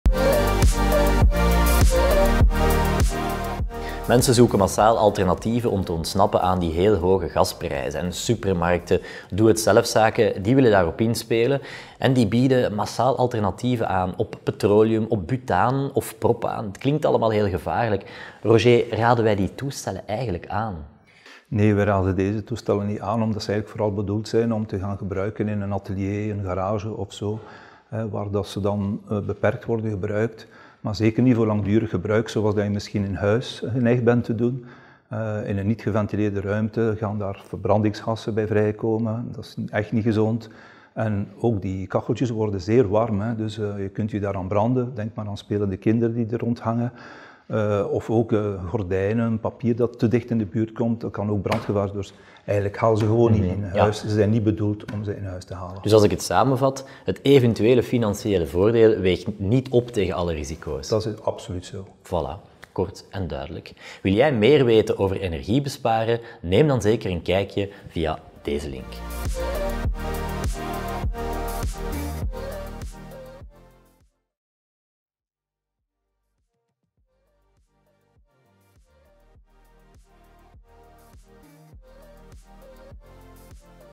Bijverwarmen op petroleum of gas, is dat wel veilig?
Petroleumproducten zijn niet zo sterk in prijs gestegen als aardgas of elektriciteit, waardoor steeds meer mensen interesse tonen voor petroleumkacheltjes en kacheltjes op propaan of butaan uit flessen. Helaas vormen die niet dé oplossing. Het blijkt moeilijk om ermee te besparen en je zet je gezondheid op het spel